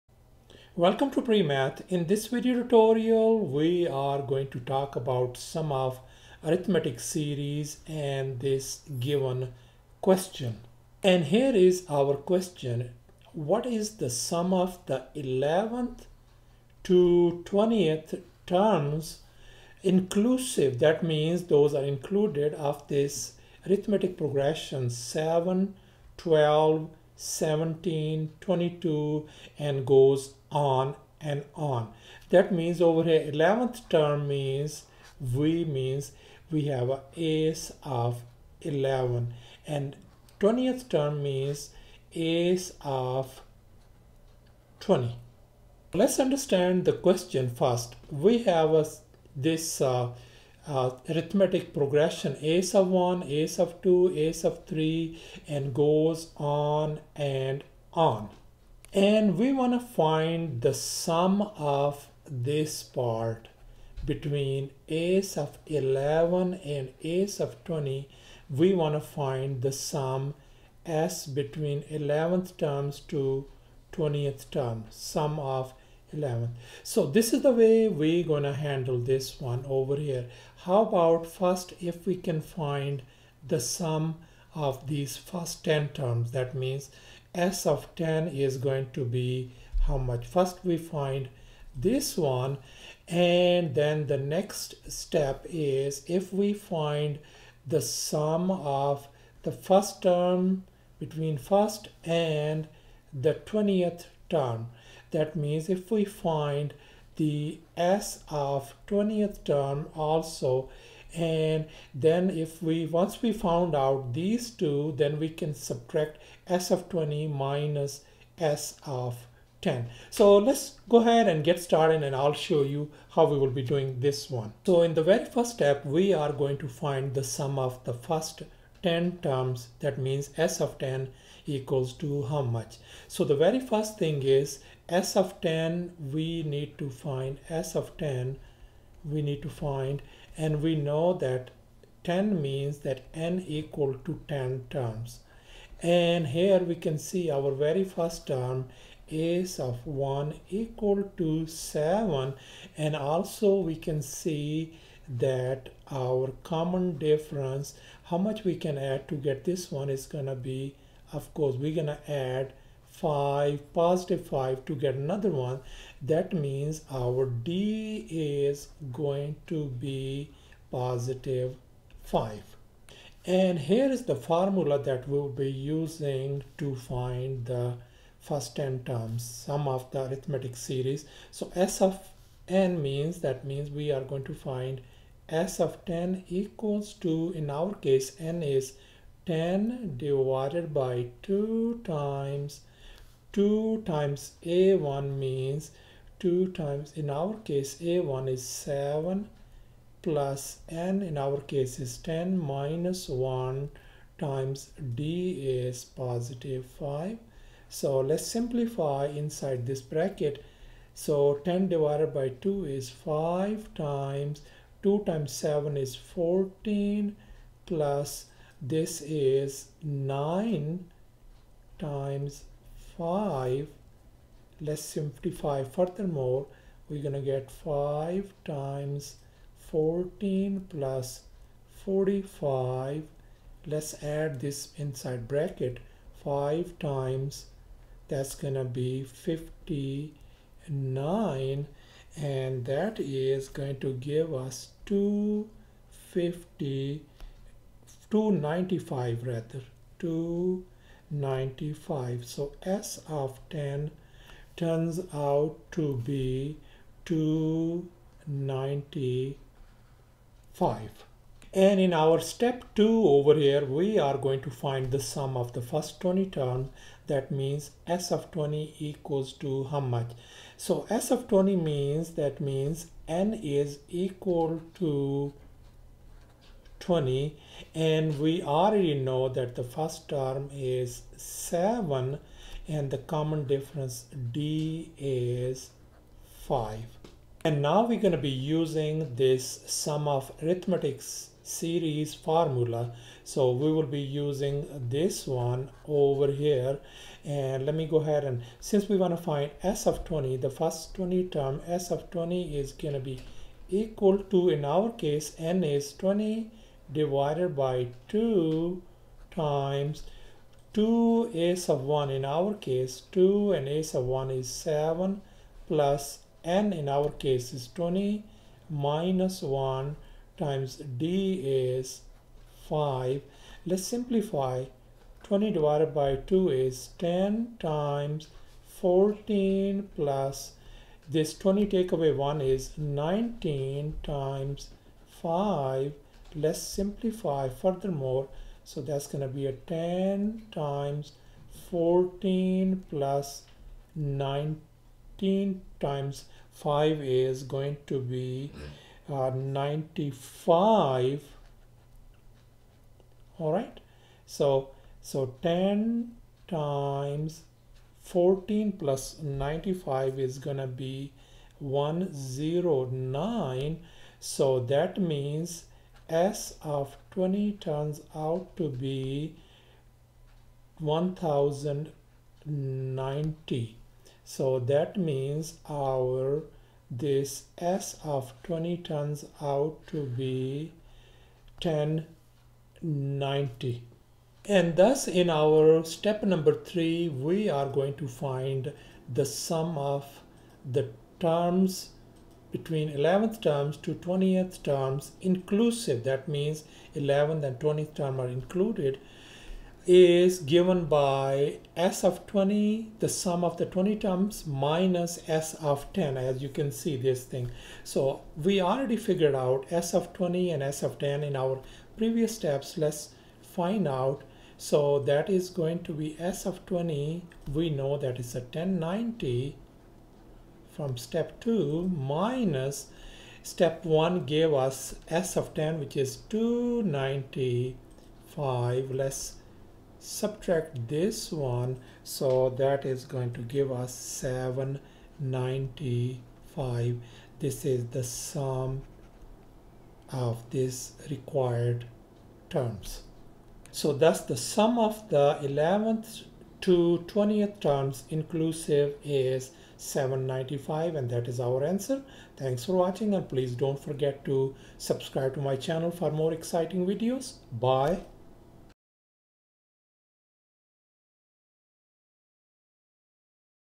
Episode 10: Find the Sum of the Eleventh to Twentieth Terms (Inclusive) of an Arithmetic Progression
Learn how to find the sum of arithmetic series (arithmetic progression sum). Do this by learning the arithmetic sequence formula in a quick and easy way. Step-by-step tutorial by PreMath.com

"What is the sum of the eleventh to twentieth terms (inclusive) of the arithmetic progression: 7, 12, 17, 22, ... ?"